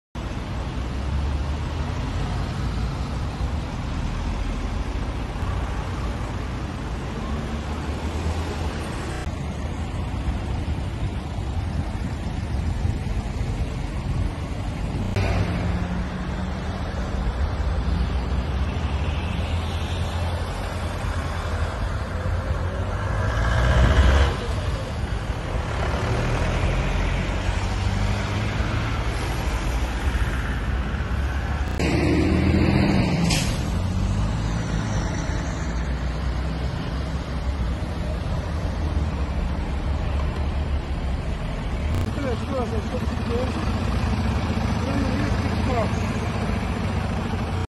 “Benzi” përplas këmbësorin në Elbasan, transportohet në gjendje të rëndë në spital
Një aksident është shënuar, mbrëmjen e sotme, në aksin Elbasan-Metalurgji. Një makinë tip “Benz” ka aksidentuar një këmbësor duke e lënë të plagosur.
Burime për “Panorama” konfirmojnë se i plagosuri, identiteti i të cilit nuk është bërë ende i ditur është transportuar në spital në gjendje të rëndë shëndetësore.